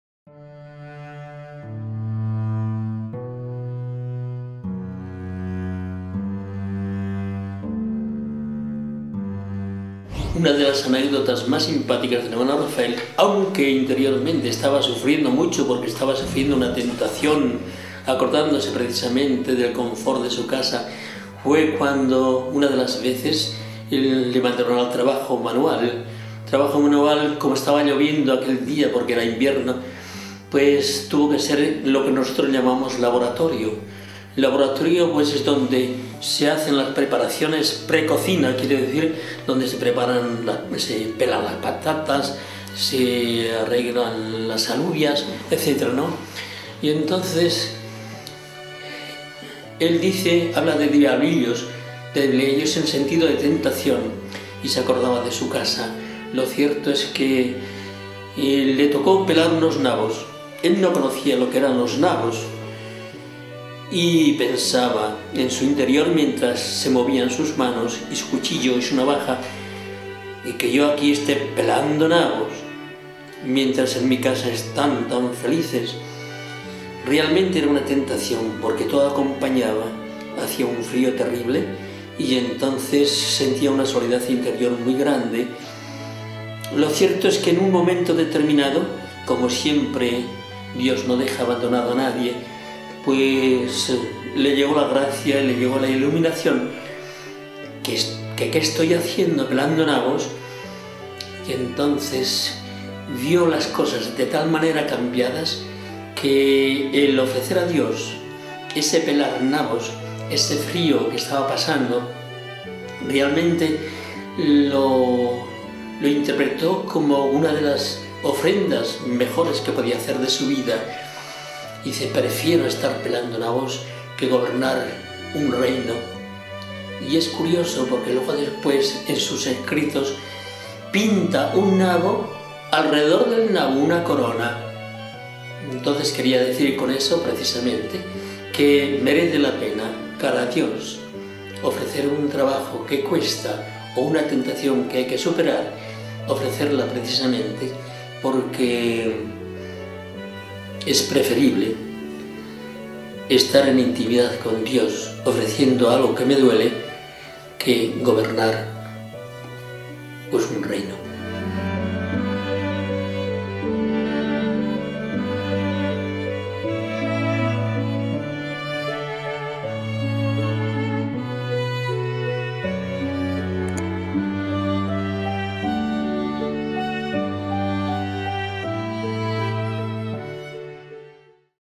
Hermano Rafael - Anécdota de los Nabos
San Rafael Arnáiz Barón, monje trapense, considerado uno de los místicos del siglo XX.
Tuvo que salir varias veces del monasterio debido a la diabetes que lo aquejaba, pero le bastó para dejar tras de sí una huella de santidad sencilla y heroica a la vez.
Murió  el 26 de abril de 1938 a los 27 años de edad.